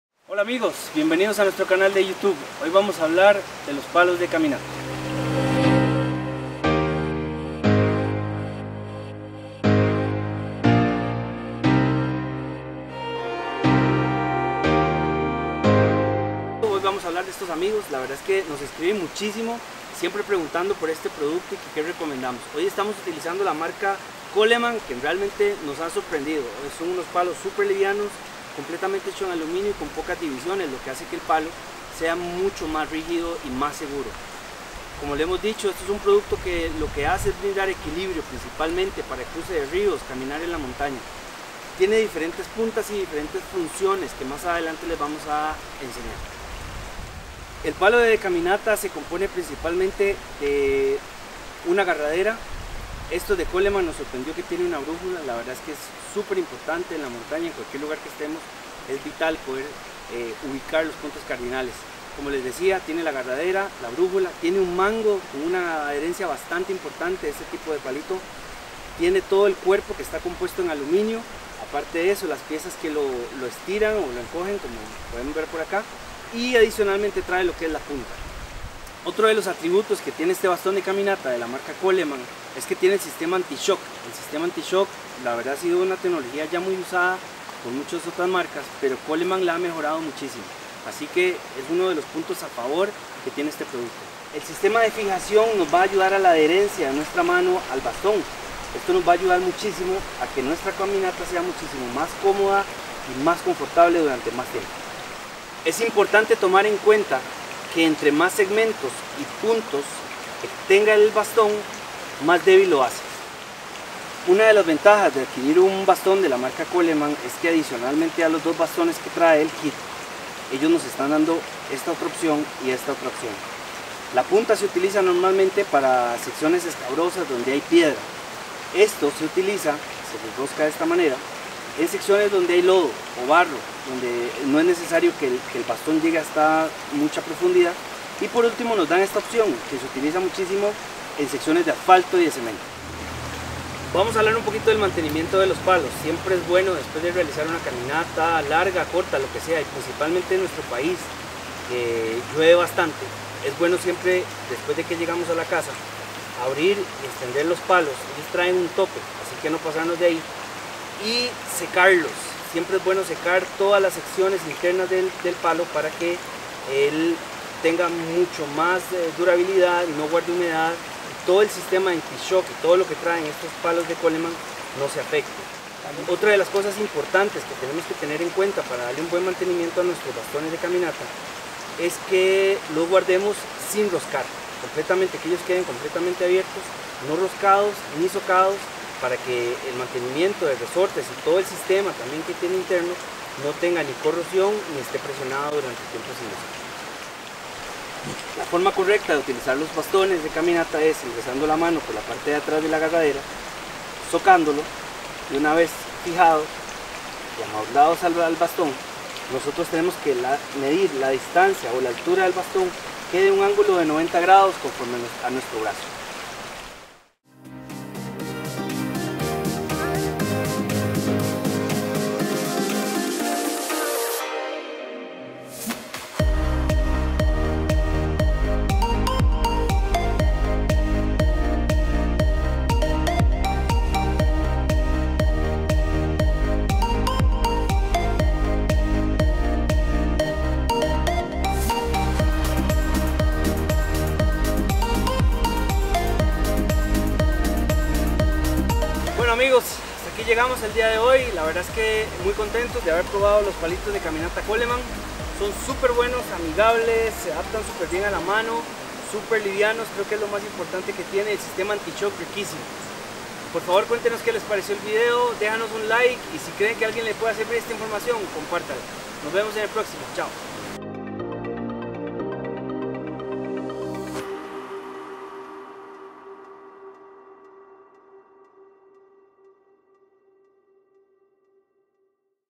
Bastones de montaña
En este video mostramos todo lo que debes saber y conocer de los bastones o palos para caminata.
Agradecemos a Coleman Costa Rica por el apoyo y soporte para realizar este video.